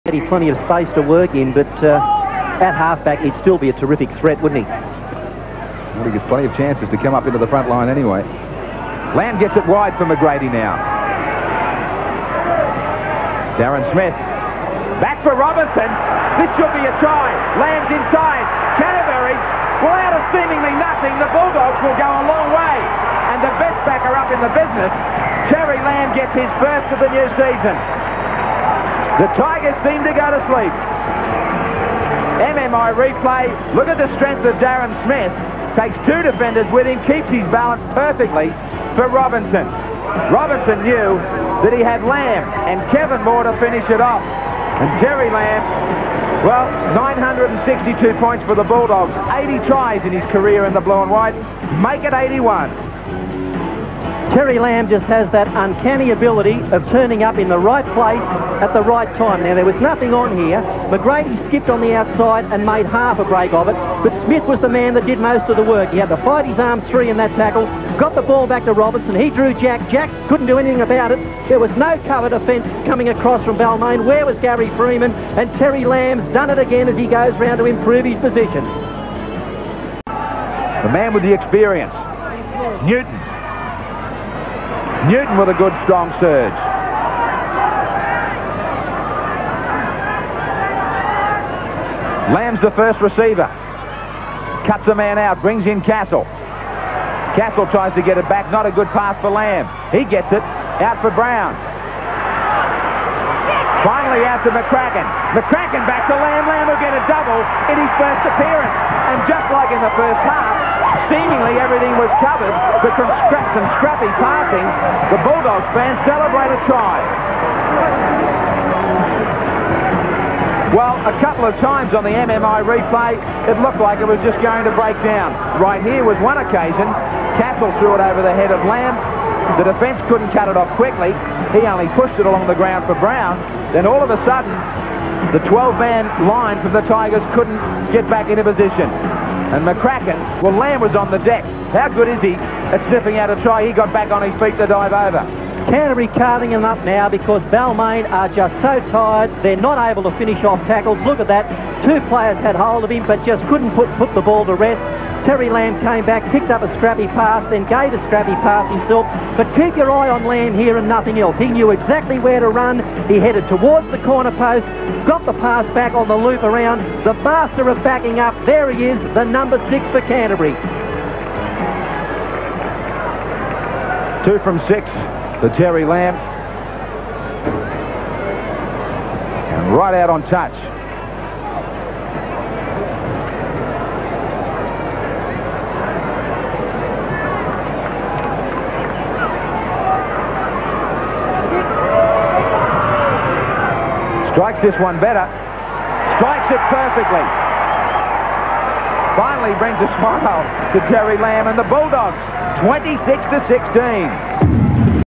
Terry Lamb Two Tries 1991 Rd 1 Canterbury v Balmain
Terry Lamb's two tries for the Canterbury-Bankstown Bulldogs against Balmain Tigers in 1991 Winfield Cup Round 1 at Leichhardt Oval. Commentary by Graeme Hughes, Bill Anderson and Wayne Pearce.